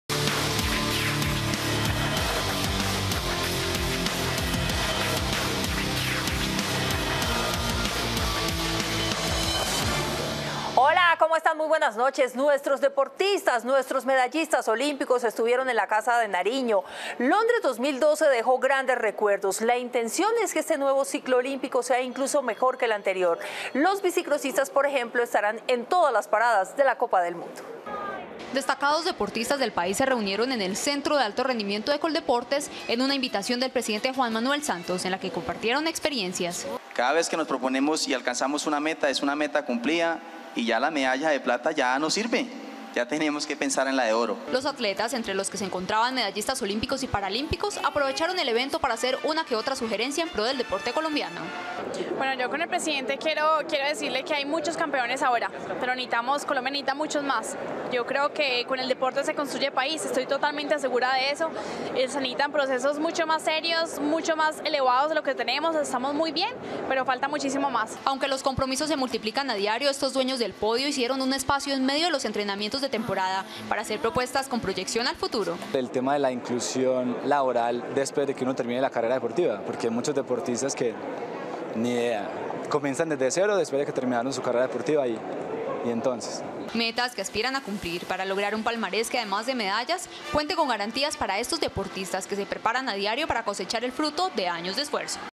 Destacados deportistas colombianos se reunieron con el Presidente Santos
Noticias Capital 8:00 p.m. enero 30